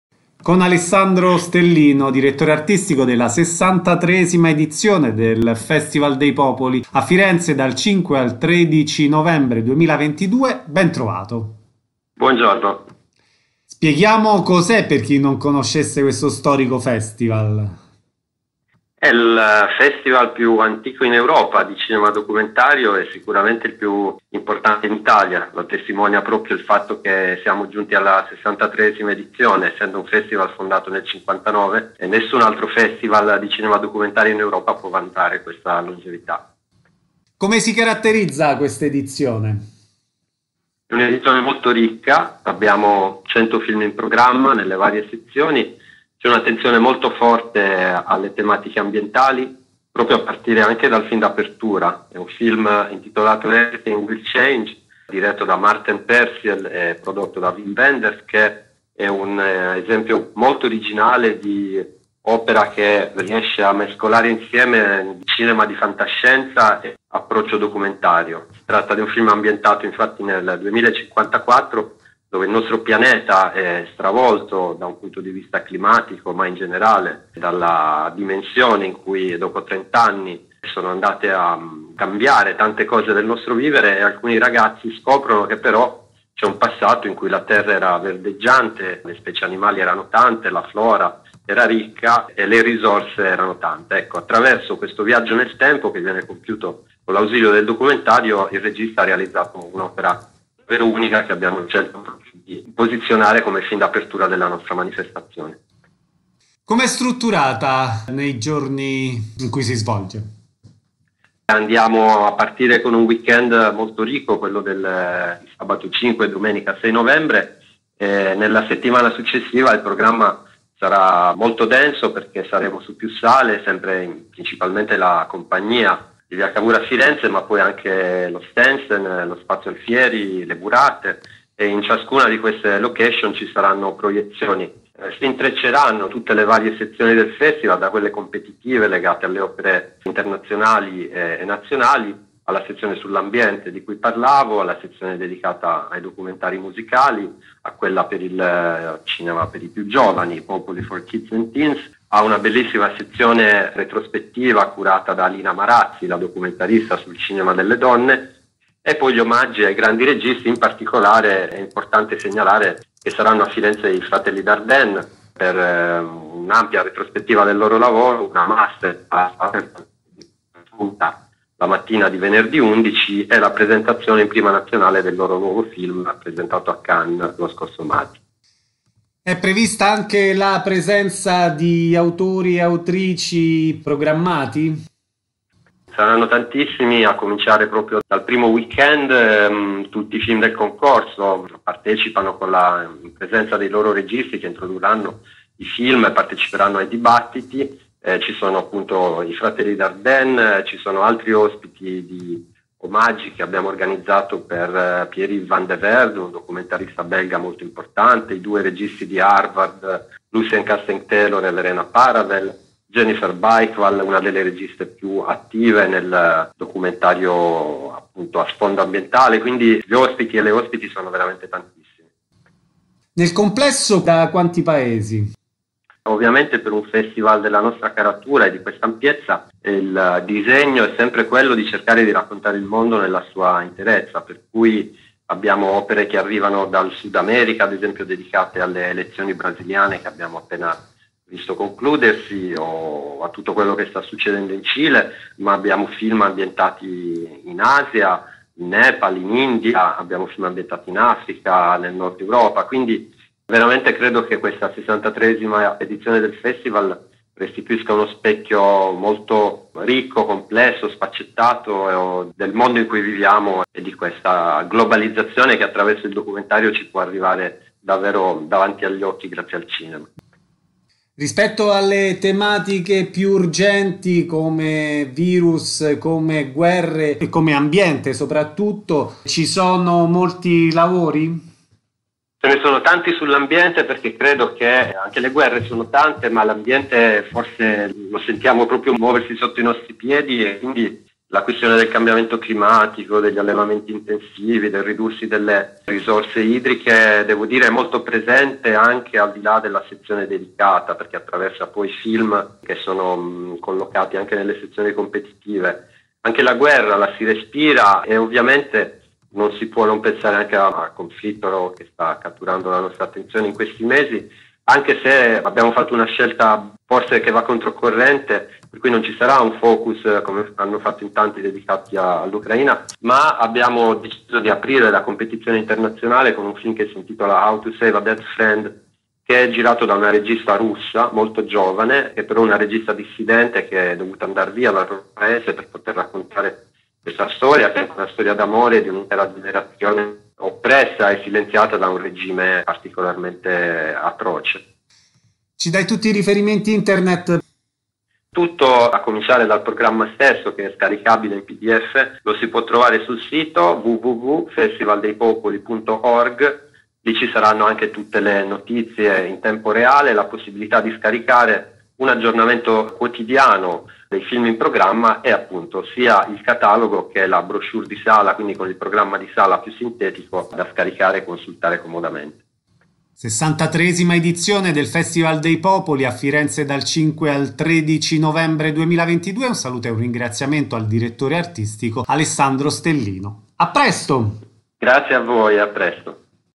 20221102 - Alessandro Stellino - fdp, 63
Alessandro Stellino, direttore artistico, presenta la 63^ edizione
FESTIVAL DEI POPOLI
Firenze, 05-13 novembre 2022